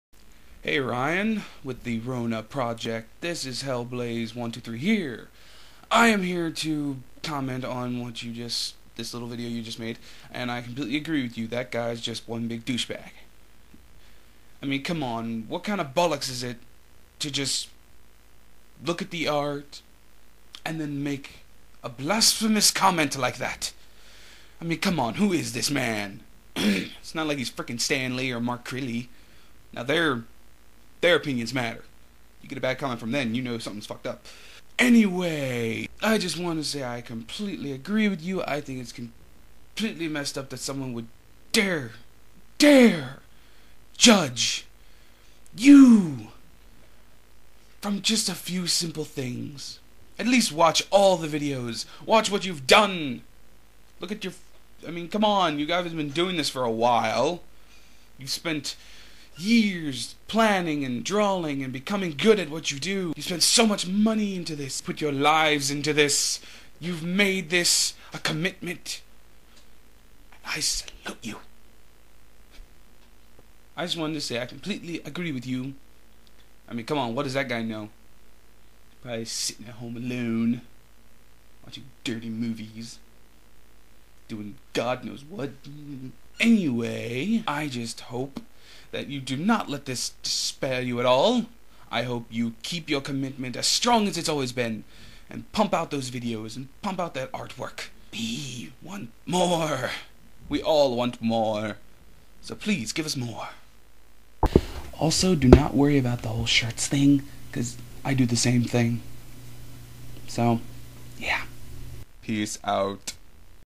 Re: Dont Judge A Book By Its Cover!!!!!
This is my response to RonaProject's newest video.

Also, Ryan, i choose TP over clothes too!